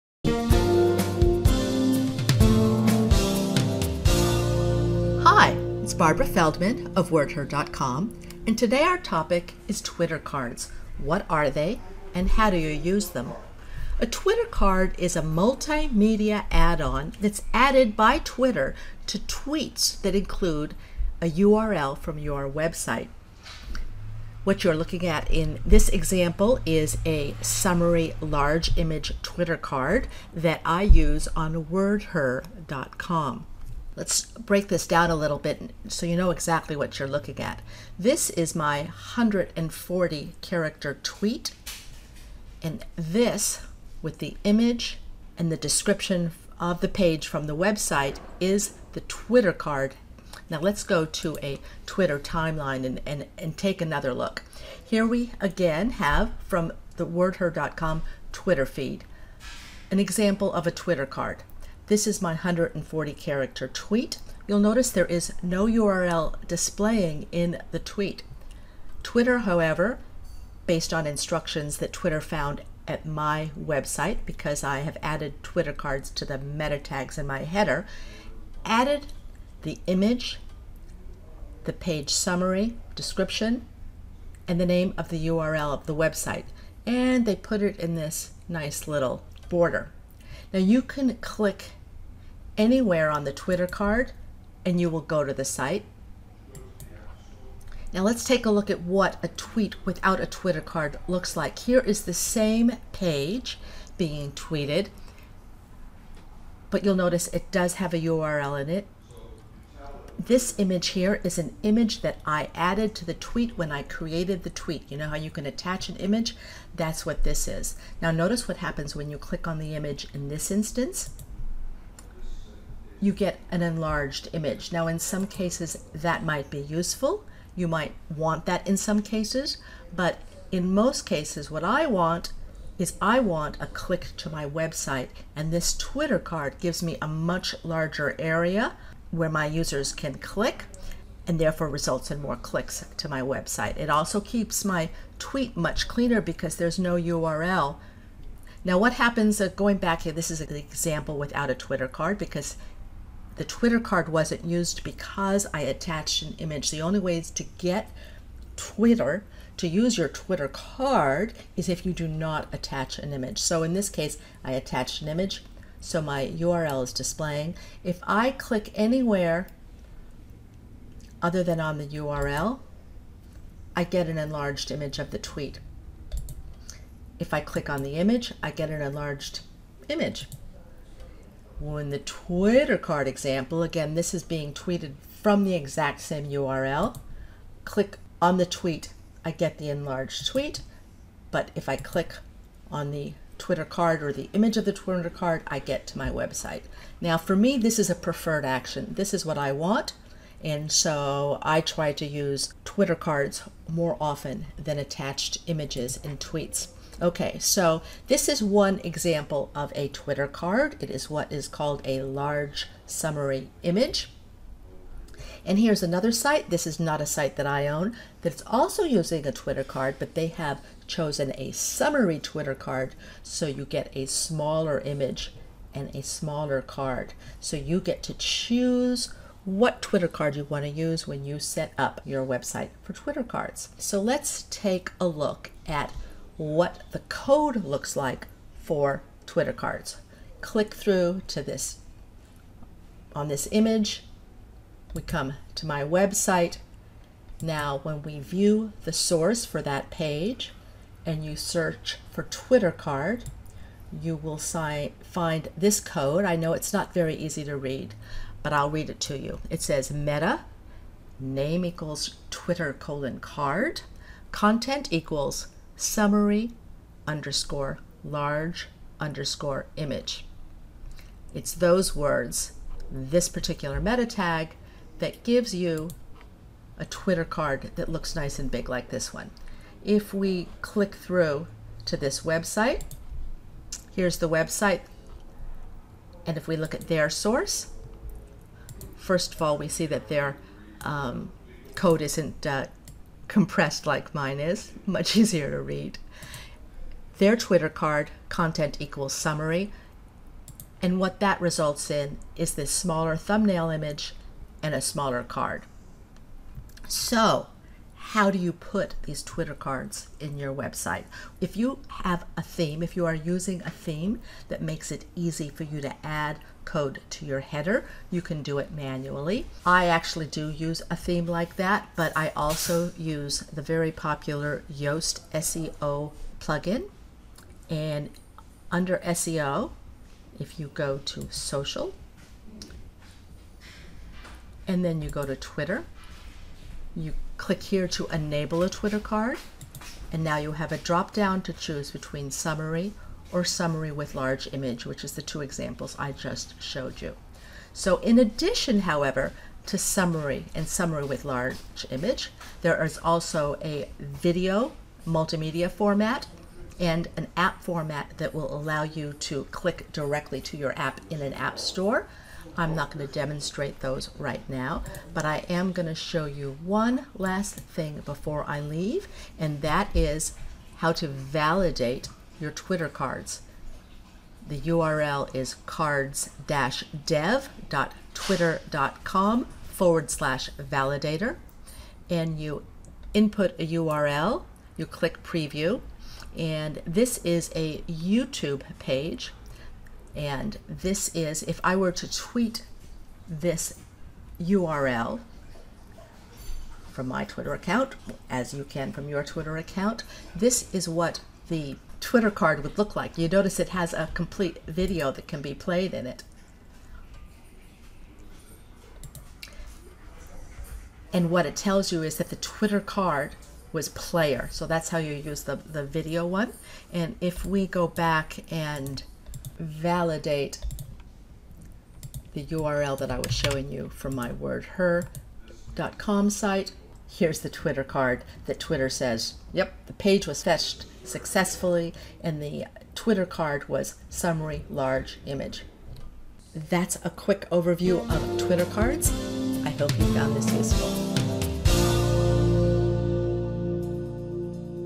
What is a Twitter Card?
What is a Twitter Card?  In this video, I explain what a Twitter Card is, show you how different kinds of Twitter Cards are displayed, explain when Twitter Cards are seen, demonstrate how I add Twitter Cards to my sites (it’s super easy), and how to validate Twitter Card meta tags once they’re added to your site.